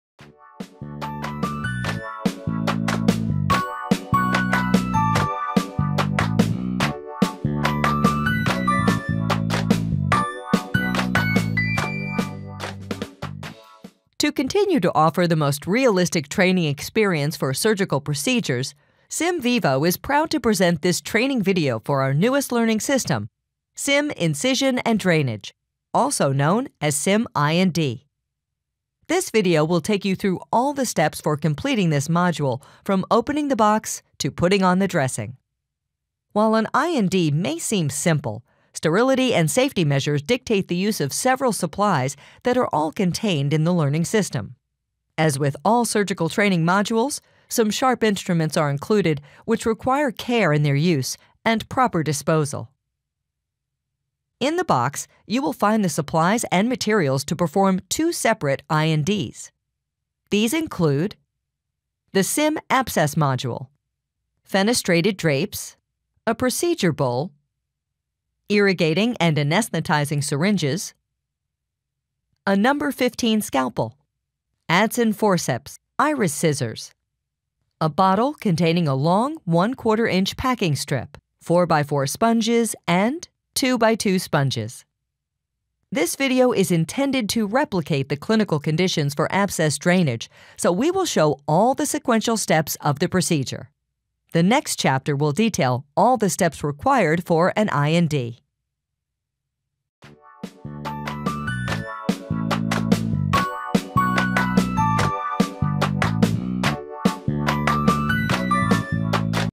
Sim*Incision & Drainage: Chapter 1, Introduction and Contents
This brand-new personal learning system includes everything to create a high-fidelity learning environment for incision and drainage. From start to finish, there isn’t a more life-like scenario in a non-clinical setting. While an I&D may seem simple, sterility and safety measures dictate practice in a simulated situation.

Beginning with an evaluation of the inflammation, notice the true look and feel of a tender abscess. Once the abscess is located, you’ll drape the area and then carefully inject anesthetic using a 3mL syringe. You’ll then proceed to incise, drain, irrigate, and finally finish by packing the open wound. Sim I&D offers a truly realistic experience.

Our abscess is made with a fully proprietary blend of non-toxic material to create the texture and consistency of pus, offering unmatched realism in pop, squirt, and ooze.